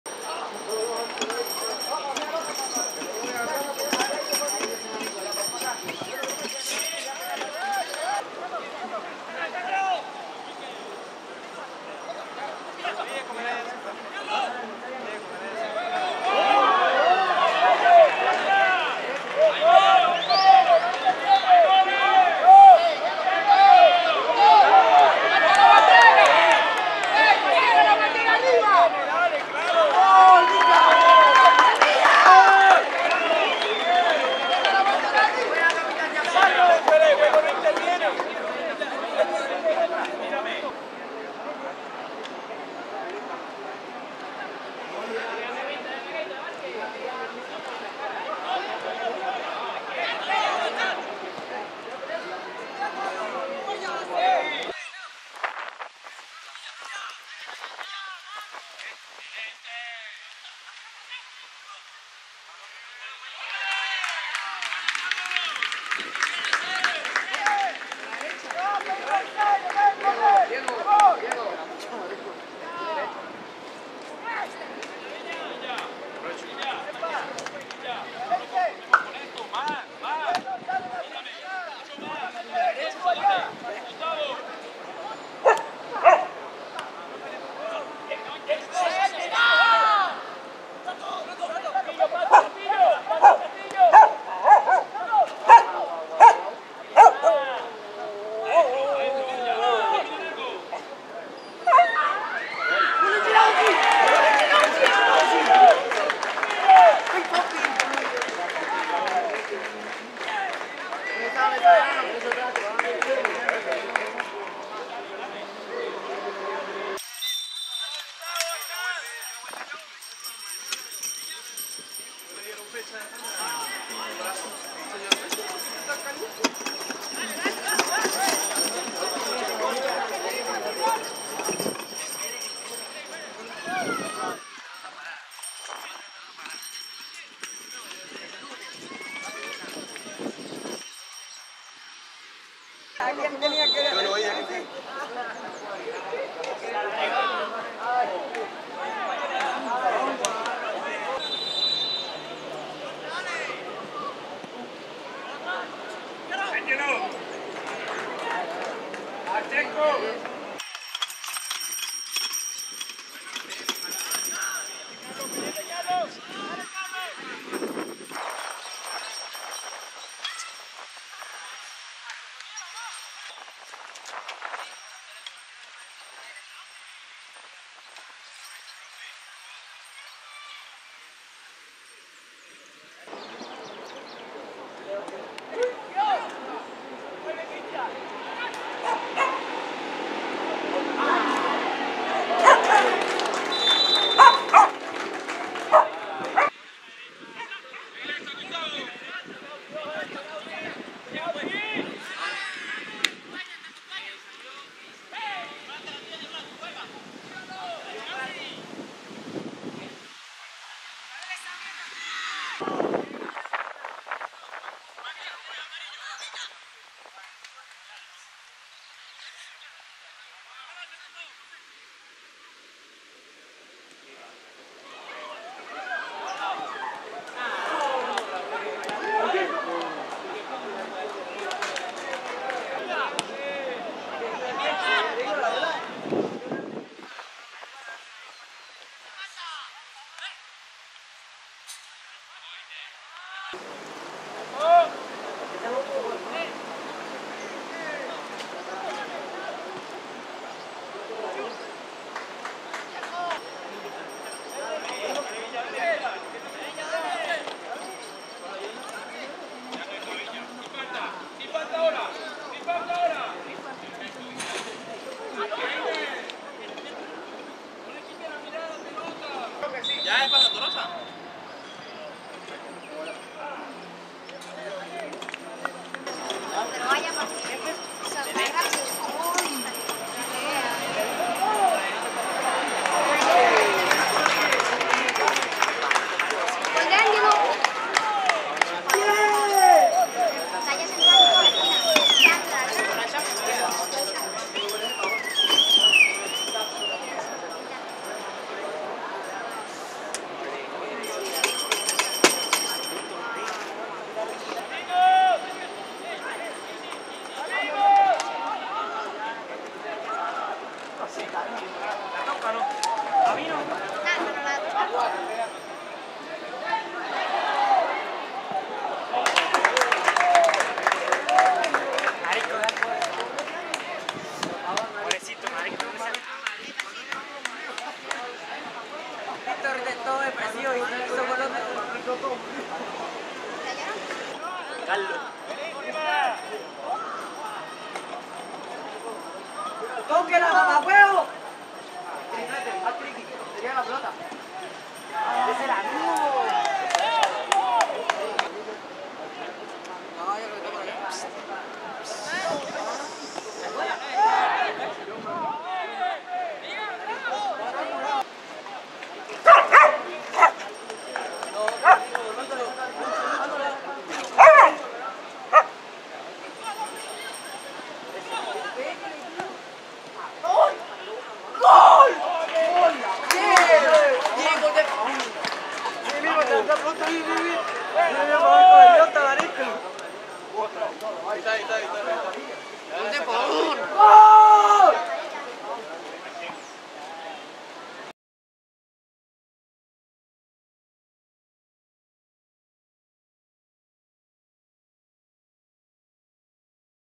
GOLEADA Sub17 | Deportivo Tachira vs El Vigia | 6-1 | Liga Futve Junior Fecha 10
Superioridad adsoluta del aurinegro al vencer 6-1 al vigia por la jornada 10 del clausura Liga Futve Junior 2024 sub 16

Goles:
Deportivo Táchira: Santiago Villamizar, Carlos Verenzuela 14’, Cristian Echenausi 19, Ángelo Medina al 81’ y 83’, Víctor Pabón 90.

El Vigia:  José Valero 37’

CR Video GOL Videos Scouting Promoción
*Promocionado tus Talentos*